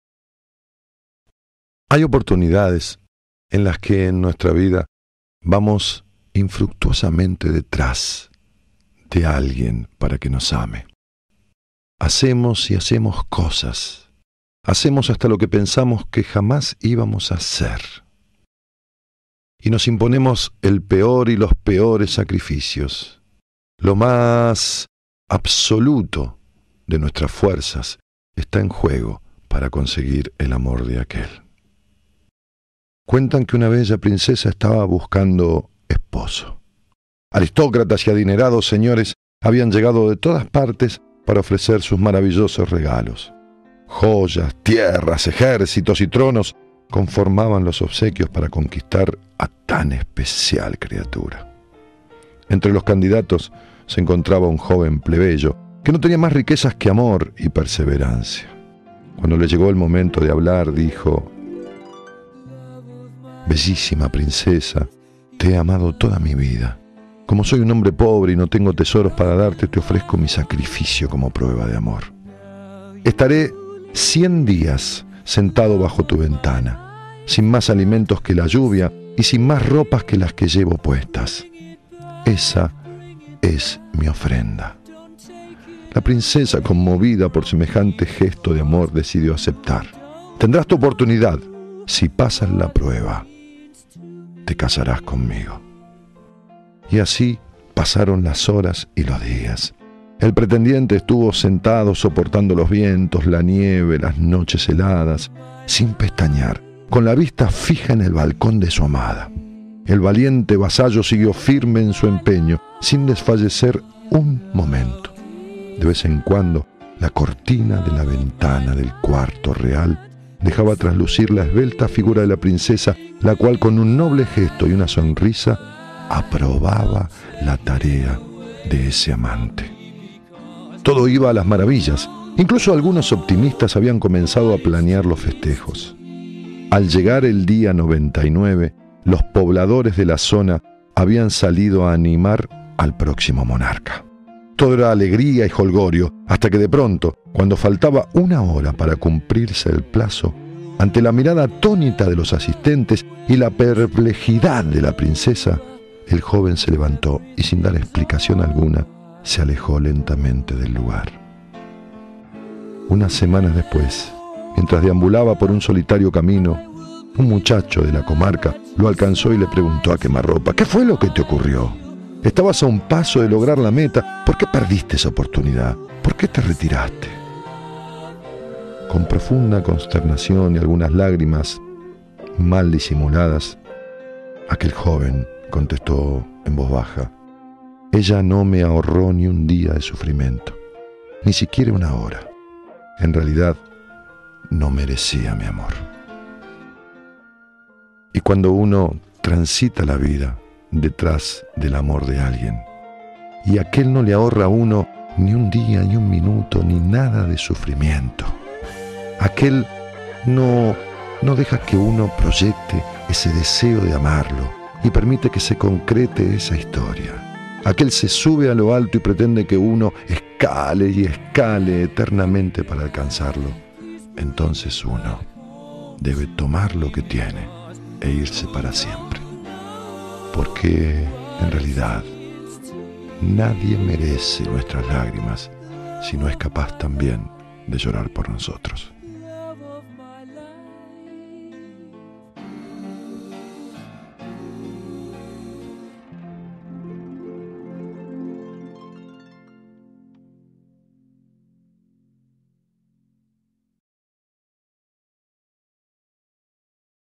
lecciones de vida para toda la vida 02
muchas veces hacemos hasta lo imposible por ser reconocidos por la persona a quien queremos, vale la pena el sacrificio nuestro ? escuchen estas palabras